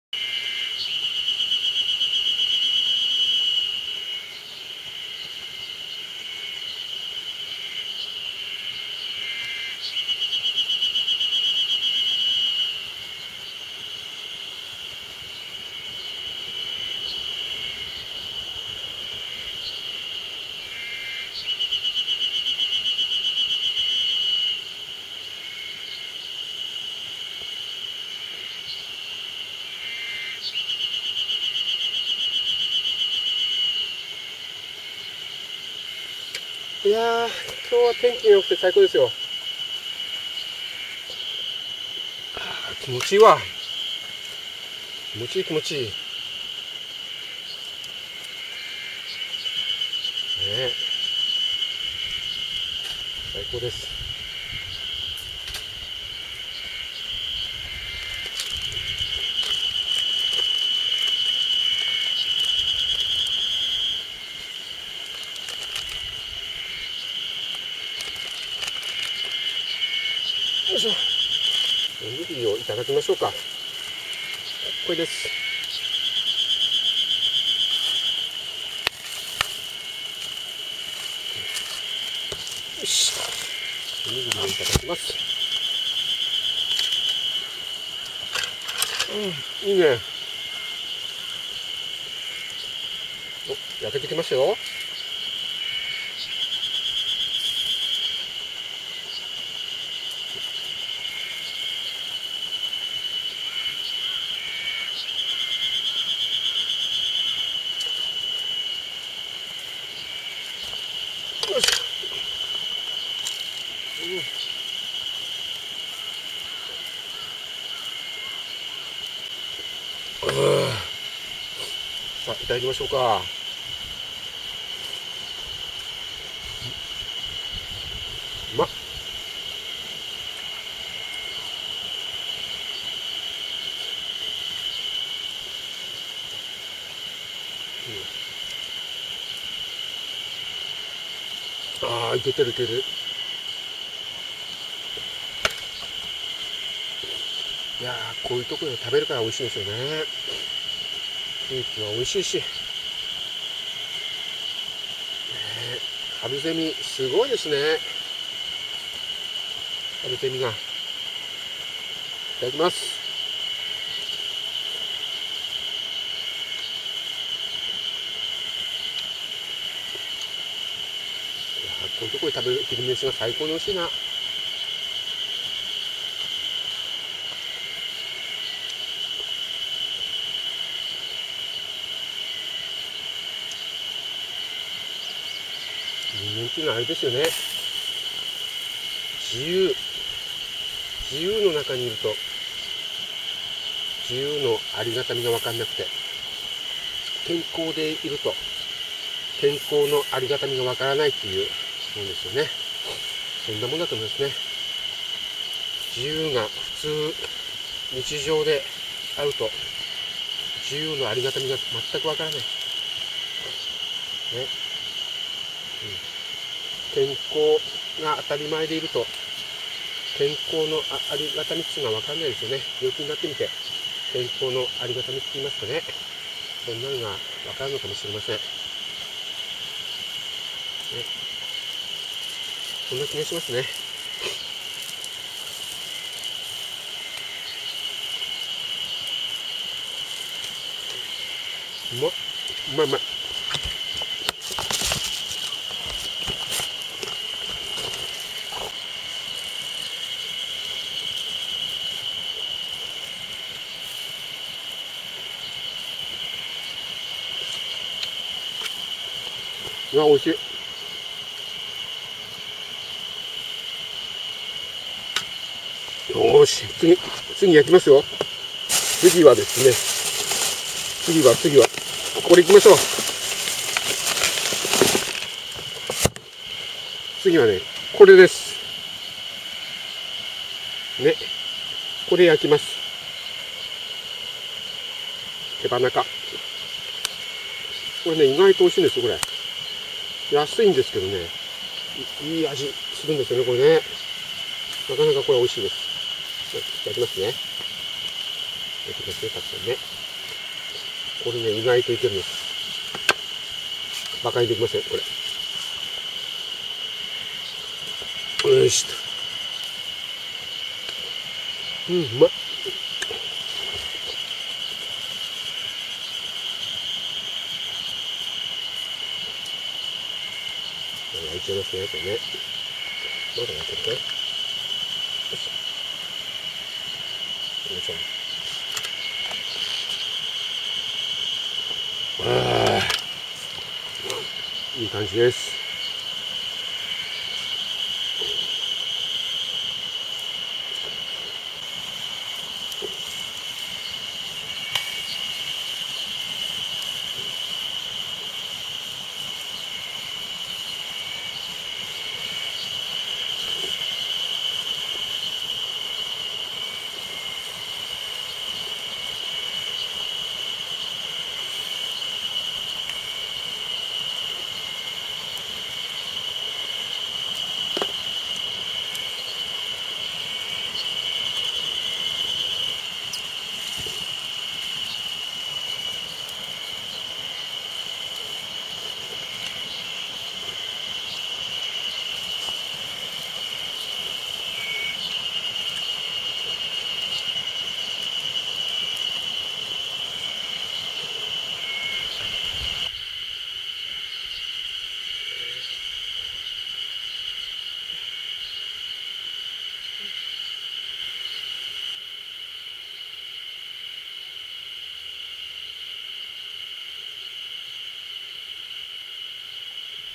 初夏の北海道十勝 ★最高の休日のひと時★ 昼はこんな感じで楽しみます。
初夏を迎え、どの川に入ってもたくさん釣れてくれます。初夏のひと時を思いっ切り楽しみます。